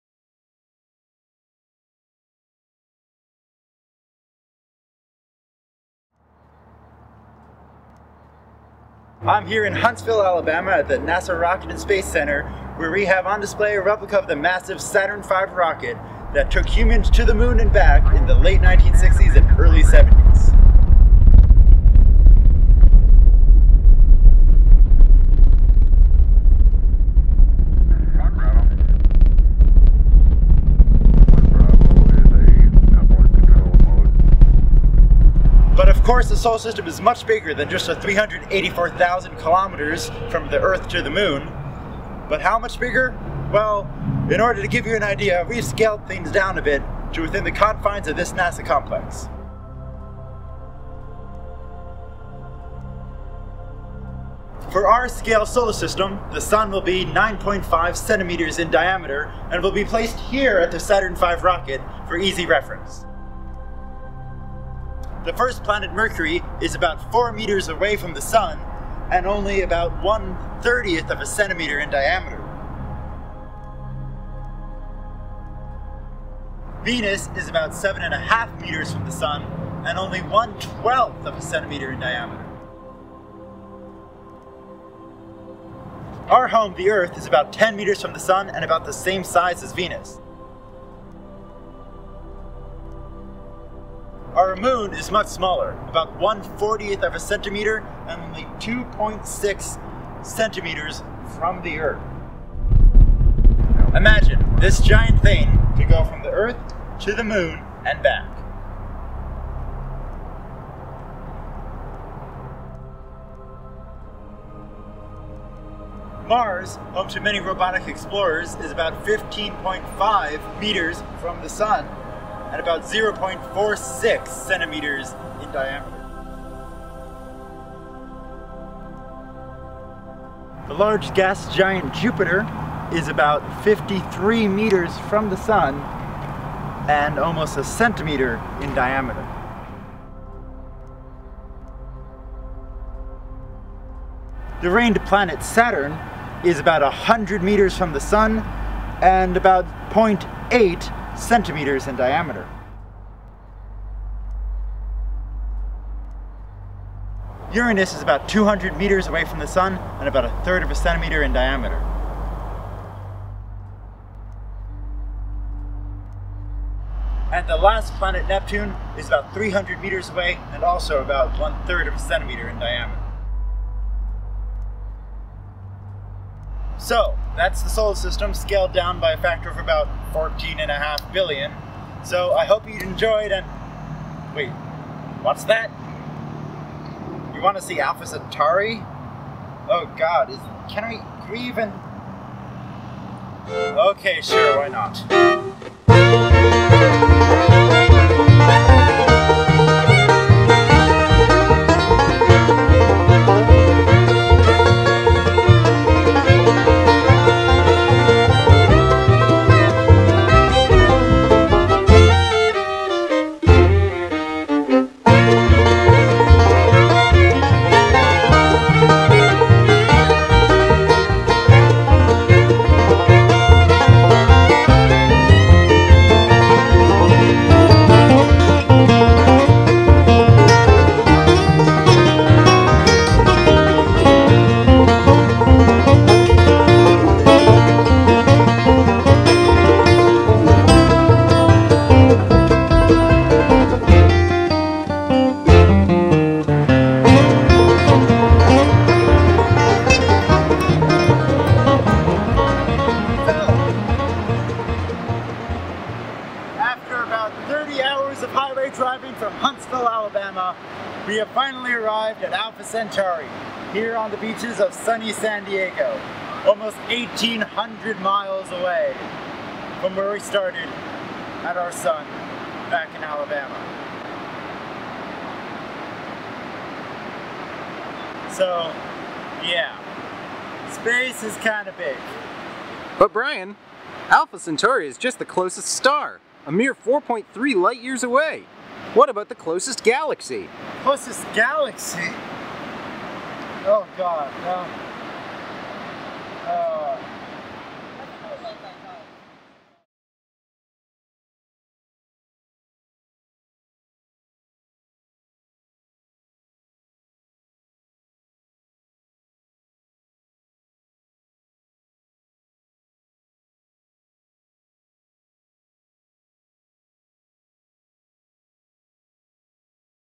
A Scale Model of our Solar System
The NASA Space and Rocket Center in Huntsville, AL provides a great opportunity to see just how big space is... and how small we are.

This is one of my first attempts at a educational science video, filmed during a road trip across the country. Unfortunately I only came up with the idea after the road trip had begun, so I had to make do with what I had on hand. Still, I think the video is based on a neat idea, and I hope you enjoy it.